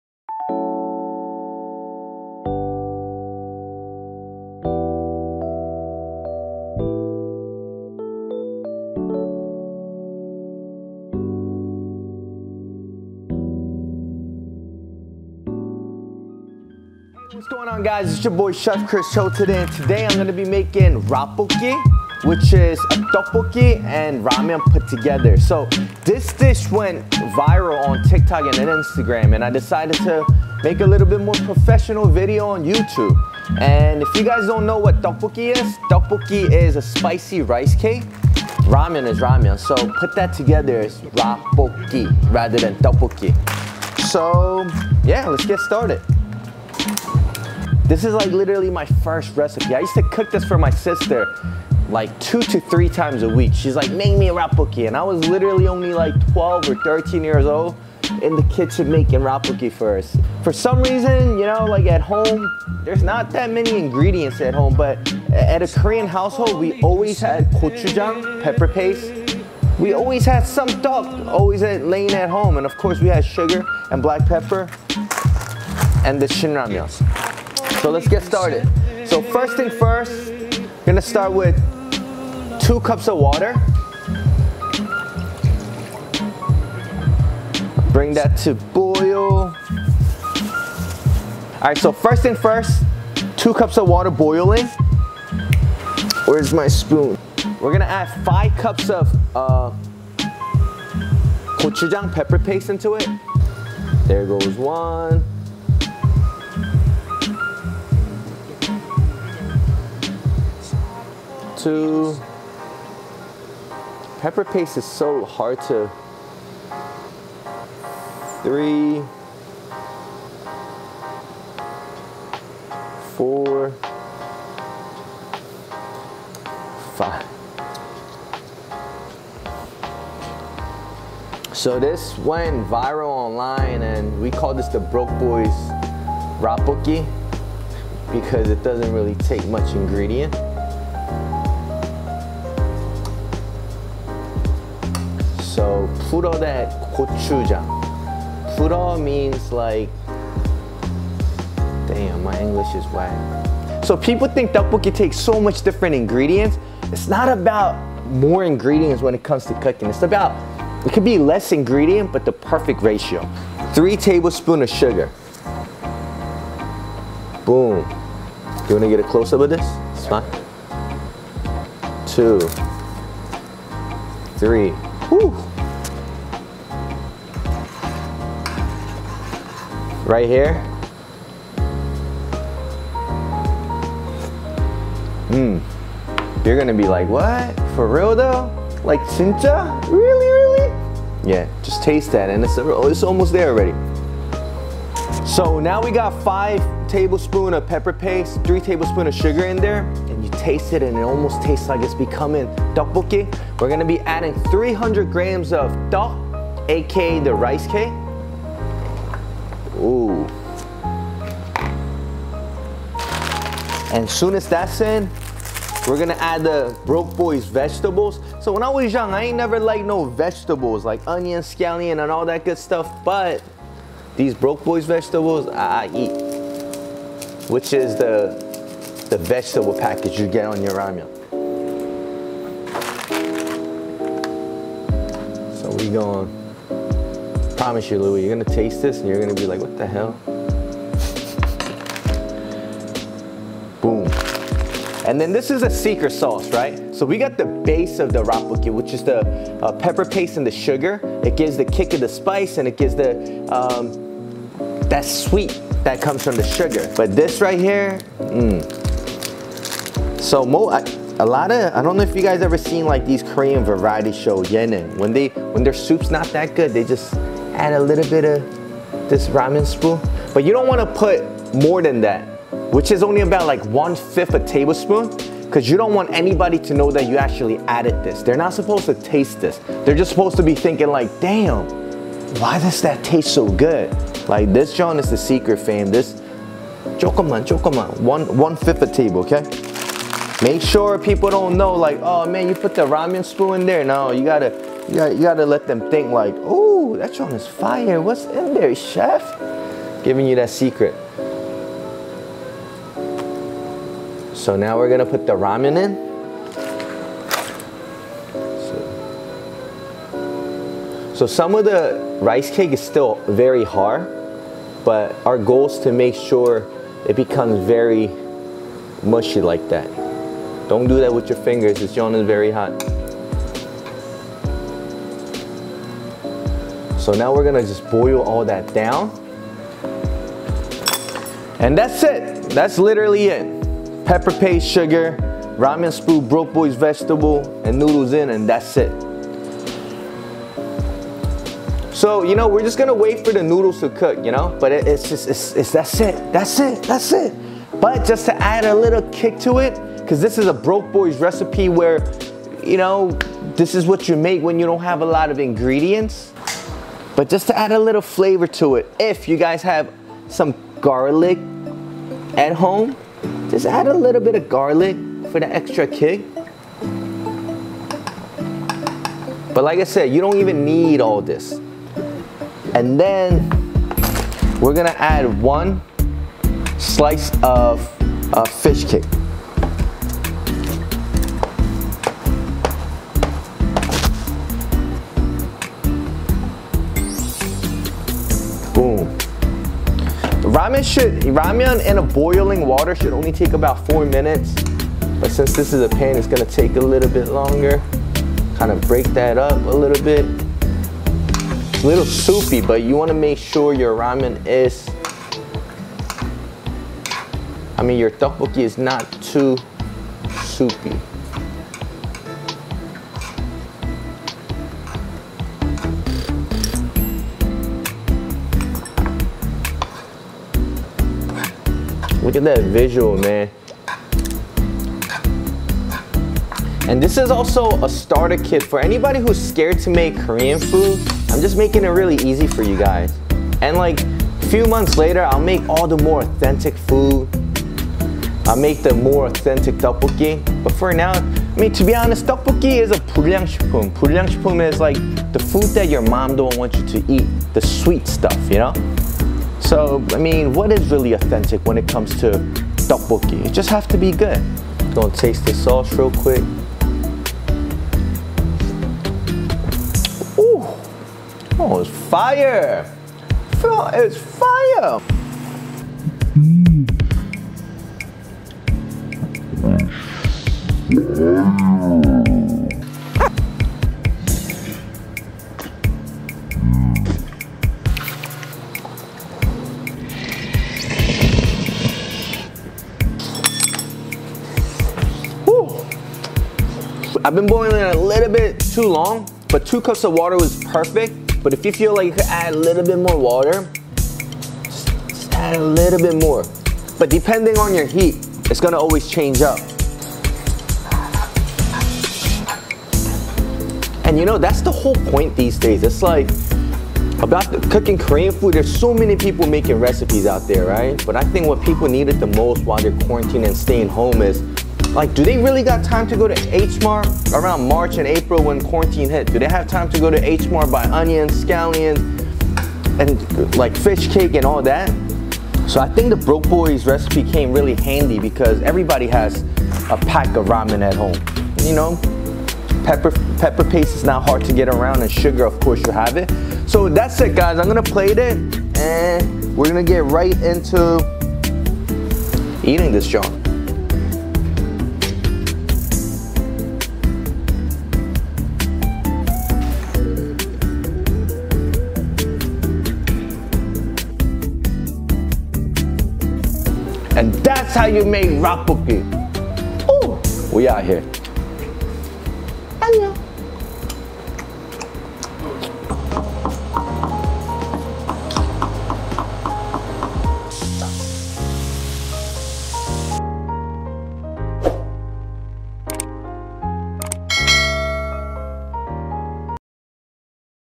Quickest RaBokki (Ramen + Tteokbokki) aka Spicy Rice Cake (라볶이)
This is truly one of my favorite recipes growing up. Actually, it's my FIRST ever recipe I made when I was 12-13 years old. I used to make this for my sister at least ONCE a week. I guess this is where Broke Boyz Recipe started from! =)

Recipe:
2 cup - Water
300 - Rice cake
3-4 TBSP - Pepper Paste (Depending on your spice level)
2-3 TBSP - Sugar (Depending on your sweet level)
Broke Boyz Vegetables
1/4 TBSP - Ramen Pacakge